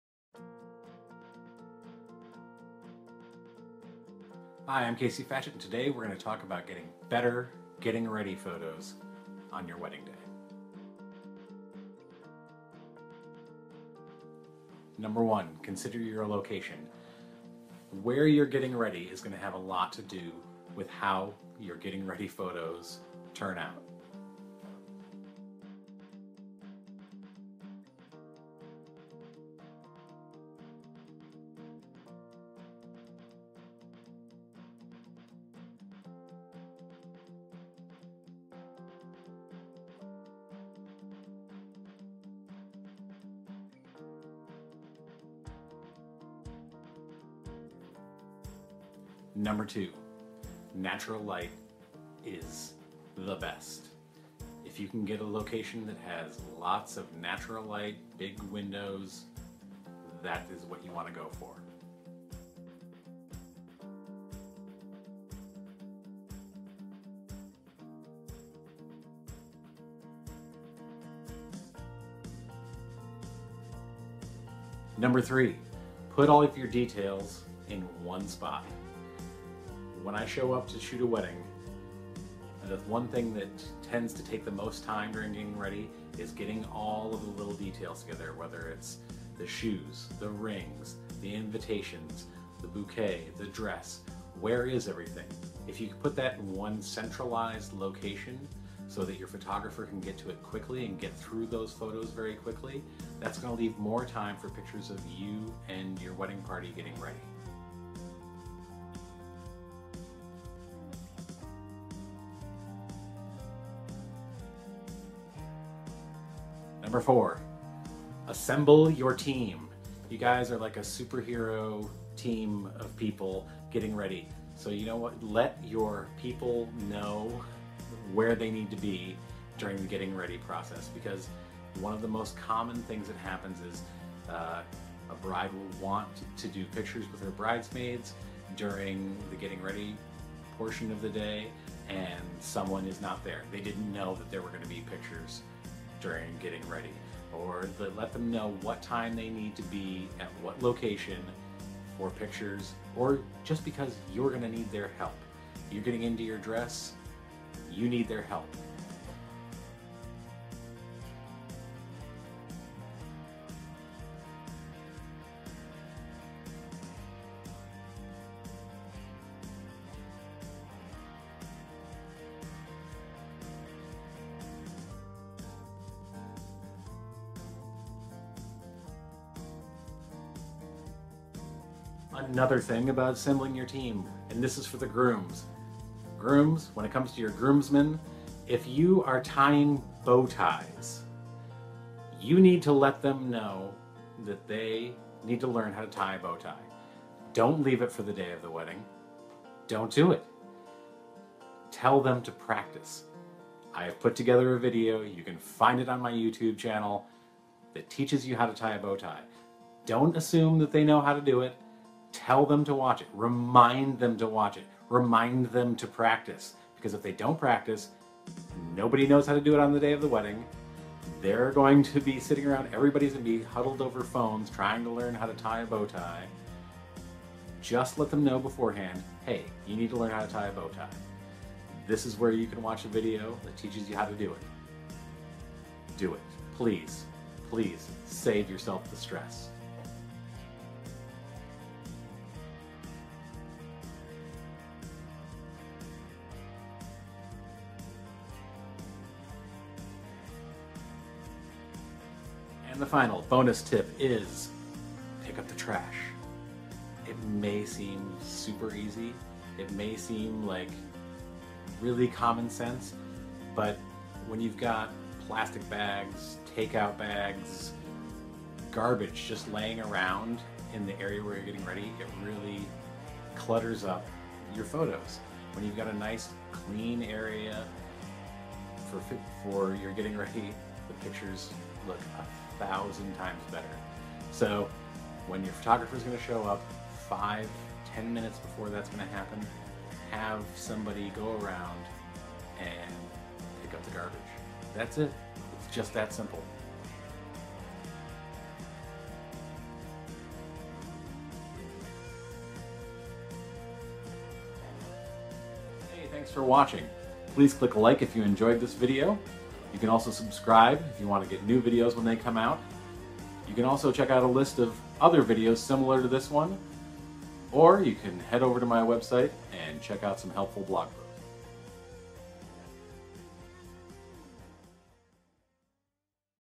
Better Getting Ready Photos
How to improve your 'getting ready' photos on the day of your wedding!